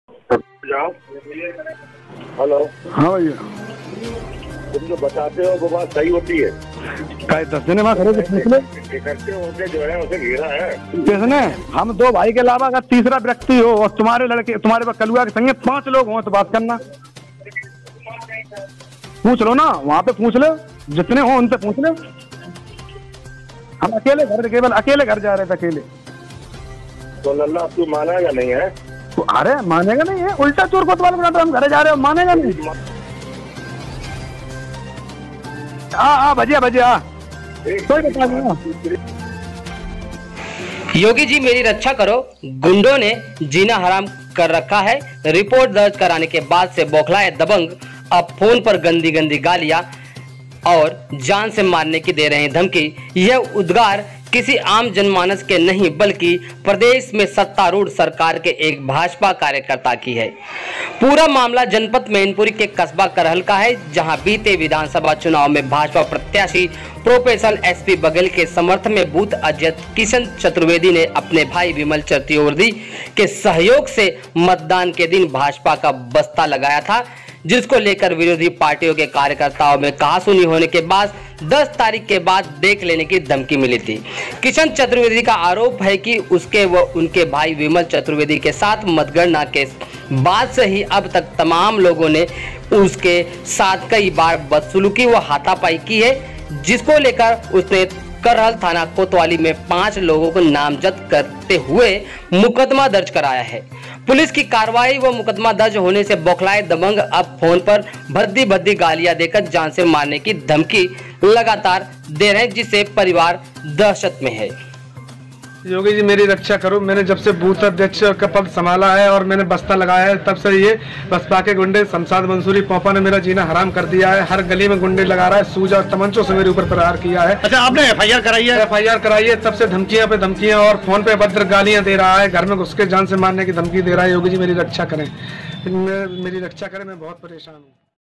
"योगी जी मेरी रक्षा करो" करहल में भाजपा कार्यकर्ता ने लगाई गुहार
जिला मैनपुरी यूपी
रिपोर्टर रामकिशोर वर्मा

स्लग : *"योगी जी मेरी रक्षा करो"* करहल में भाजपा कार्यकर्ता ने लगाई गुहार/ दबंग की दबंगई से परेशान हैं भाजपा कार्यकर्ता

एंकर : योगी जी मेरी रक्षा करो गुंडों ने जीना हराम कर रखा है रिपोर्ट दर्ज कराने से बौखलाये दबंग अब फोन पर गन्दी गालियां बक जान से मारने की धमकी दे रहे है यह उदगार किसी आम जनमानस के नहीं बल्कि प्रदेश में सत्तारूढ़ सरकार के एक भाजपा कार्यकर्ता की है
पूरा मामला जनपद मैनपुरी के कस्बा करहल का है जहां बीते विधानसभा चुनाव में भाजपा प्रत्याशी प्रोफेसर एसपी बघेल के समर्थन में बूथ अध्यक्ष किशन चतुर्वेदी ने अपने भाई विमल चतुर्वेदी के सहयोग से मतदान के दिन भाजपा का बस्ता लगाया था जिसको लेकर विरोधी पार्टी के कार्यकर्ताओं से कहासुनी होने के बाद 10 तारीख के बाद देख लेंगे की धमकी मिली थी किशन चतुर्वेदी का आरोप है कि उसके व उसके भाई विमल चतुर्वेदी के साथ मतगणना के बाद से अब तक तमाम लोगों ने उसके साथ कई बार बदसलूकी व हाथापाई की है जिसको लेकर उसने करहल थाना कोतवाली में पांच लोगों को नामजद करते हुए मुकदमा दर्ज कराया है पुलिस की कार्रवाही व मुकदमा दर्ज होने से बौखलाए दबंग अब फोन पर भद्दी भद्दी गालियां देकर जान से मारने की धमकी दे रहे हैं भाजपा कार्यकर्ता का परिवार दहशत में है वहराहल परेशान भाजपा कार्यकर्ता ने प्रदेश मुख्यमंत्री योगी आदित्यनाथ को प्रार्थना पत्र एवं टवीटर पर मदद करने की गुहार लगाई है आइए सुनते हैं इस विषय पर भाजपा कार्यकर्ता ने क्या कुछ कहा है

बाइट:--- विमल चतुर्वेदी